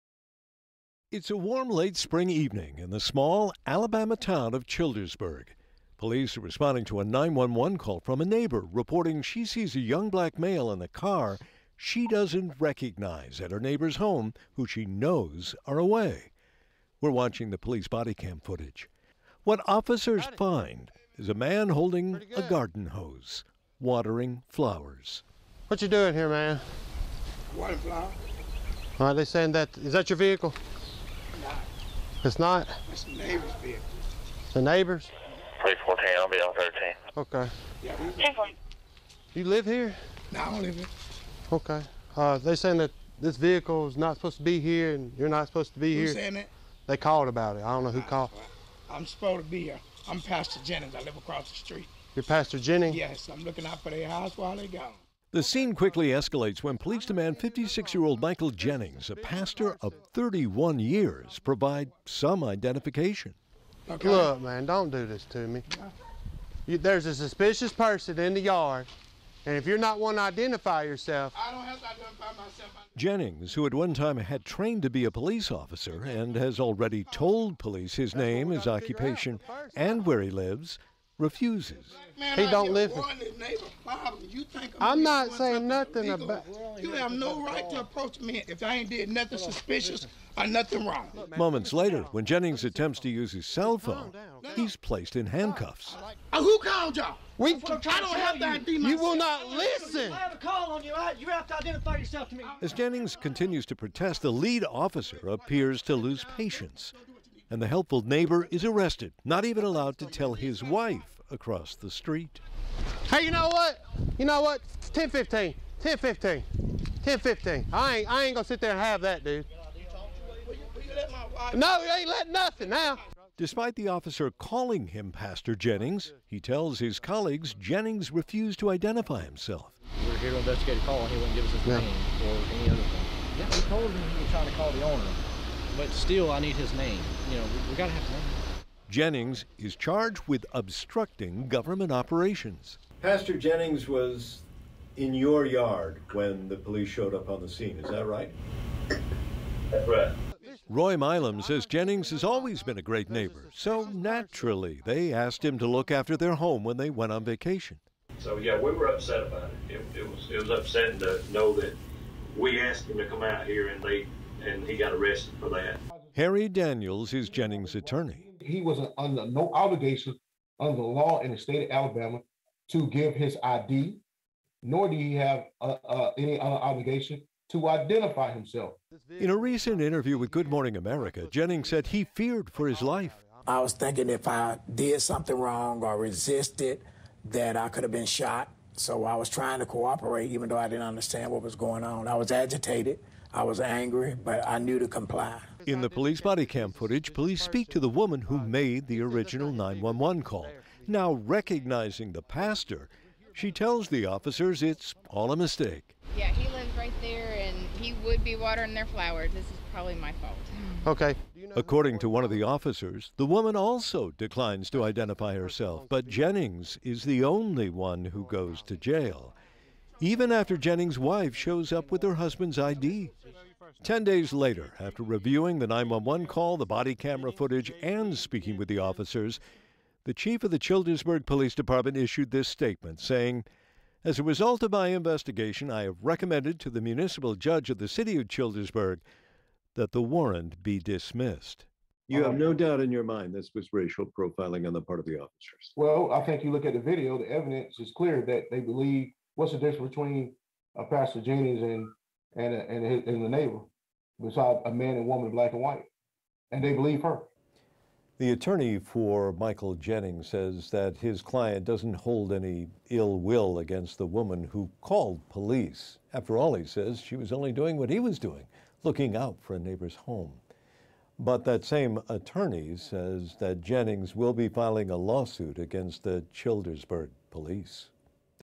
Video: Black pastor arrested while watering neighbor's flowers
A Black pastor in Alabama says he was arrested while watering his neighbor's flowers -- and his attorney has called the incident "irrational, irresponsible, and illegal."
In police body camera footage from May and released last week, an officer is seen approaching the man, Michael Jennings, who tells the officer he has been asked to water the flowers while a neighbor is gone.
In the video that has since gone viral, Jennings, of Childersburg, Alabama, is asked by the officer what he is doing at the house and whose car is in the driveway. He says it's the neighbor's vehicle and identifies himself as "Pastor Jennings" without being asked
He was arrested for "Obstructing Governmental Operations," but charges were later dropped, according to a police statement from June 1, 2022.
Police were responding to a 911 call about a "suspicious" car parked at the residence and a person "the caller did not recognize," Richard McClelland, chief of police for the city of Childersburg, said in the statement. The caller stated that her neighbors who lived at the residence were out of town and advised that the suspicious person might be inside the residence, according to the police chief's statement.